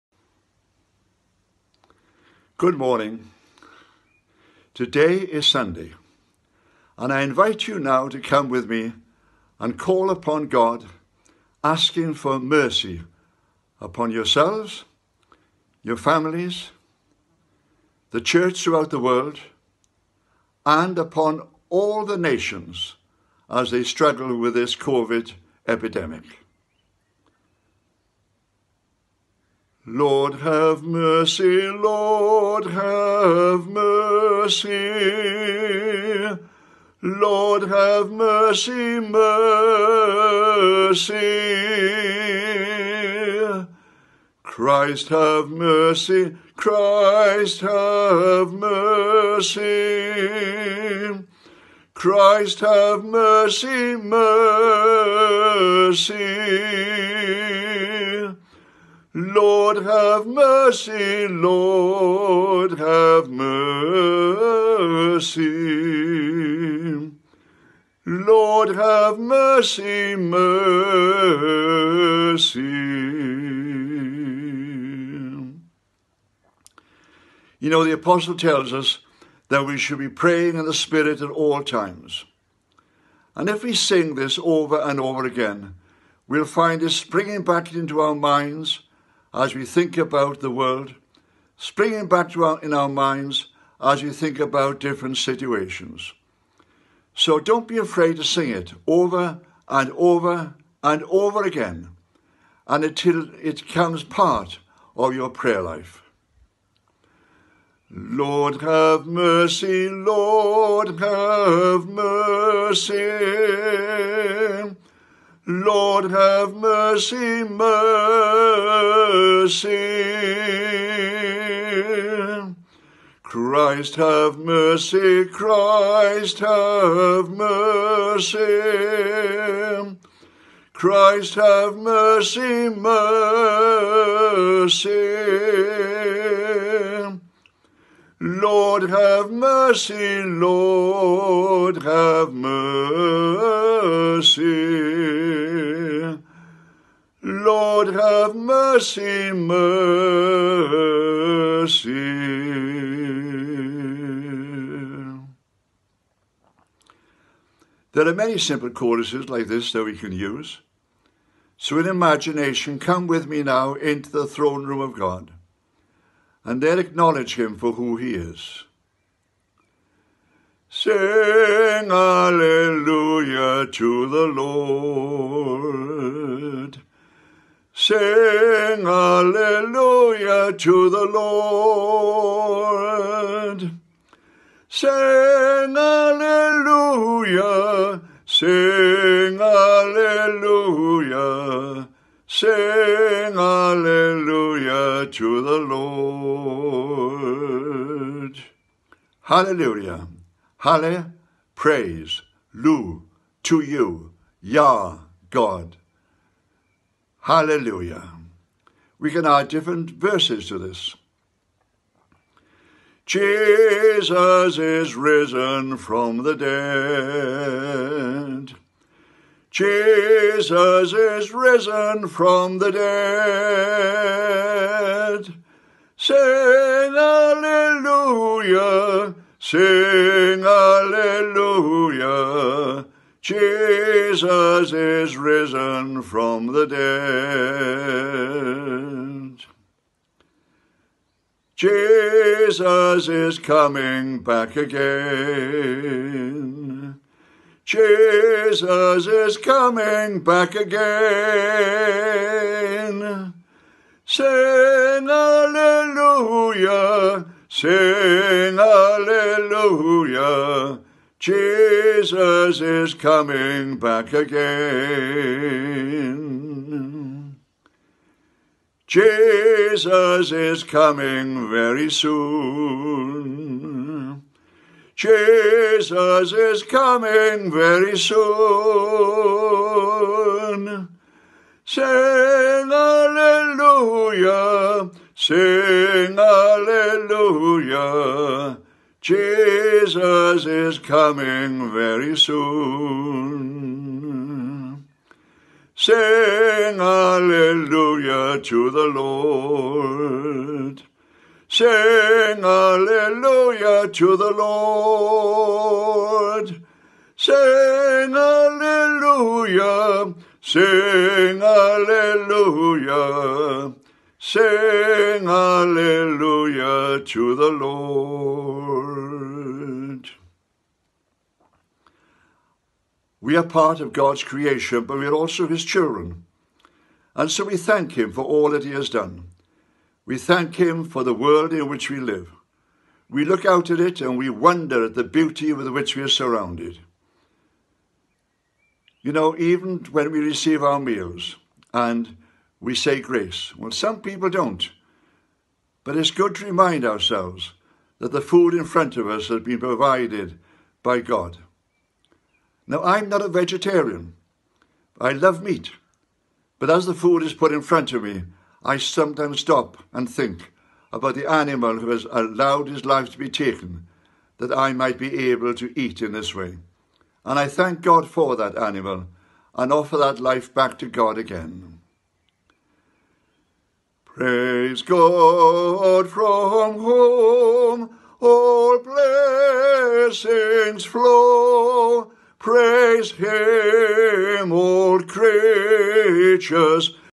2020-05-10: Fr Roy Matthews
Fr Roy Matthews invites us to use simple choruses to pray for mercy and sing praise and thanks to God.